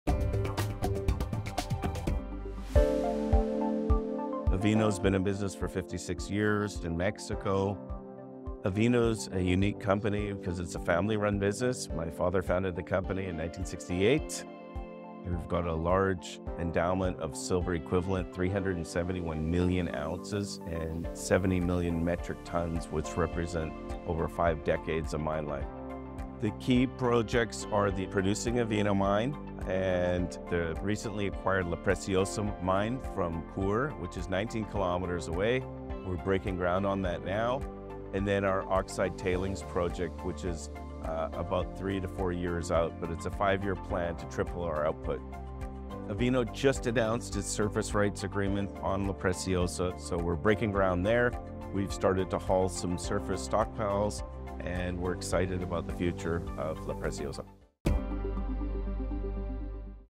Unlocking the Future of Silver: Sustainable Mining at Avino Silver & Gold Mines in Mexico
Investing in Mining Stocks

A Fresh Look at Avino's Strategic Growth and Environmental Responsibility

Avino Silver & Gold Mines has been a symbol of enduring success and sustainable growth in the precious metals sector. Founded in 1968 by the father of the current CEO, this venture has flourished into a notable silver producer, primarily operating out of its wholly-owned Avino Mine near Durango, Mexico. The company's journey from a single mine to a significant player in the silver, gold, and copper markets is a tale of vision, perseverance, and strategic foresight.

Avino stands out not only for its impressive silver equivalent reserves—amounting to 371 million ounces—but also for its commitment to environmental stewardship and community well-being. With 70 million metric tonnes of mineral resources, the company has secured over five decades of mine life, showcasing its potential for long-term sustainable and profitable mining operations.The key to Avino's strategy lies in its recent acquisition of the La Preciosa mine from Core, located just 19km away from the Avino Mine. This strategic move not only expands Avino's operational footprint but also sets the stage for a groundbreaking oxide tailings project. With plans to triple its output within a five-year timeframe, Avino is on a clear path to significantly boost its production capabilities while maintaining its unwavering commitment to environmental responsibility and the prosperity of local communities.

Avino's innovative approach to mining extends beyond its expansion projects. The company prides itself on its silver, gold, and copper production remaining unhedged, a strategy that underlines its confidence in the intrinsic value and market demand for its commodities. This bold stance, coupled with a hands-on approach to managing business activities in a cost-effective manner, has positioned Avino as a steadfast and reliable partner to shareholders and the surrounding community alike.

As Avino breaks ground on La Preciosa and advances its oxide tailings project, the future looks bright for this family-run business. With a solid foundation built on decades of experience and an ambitious plan to triple output, Avino is poised to continue its legacy of sustainable growth and environmental stewardship in the precious metals industry.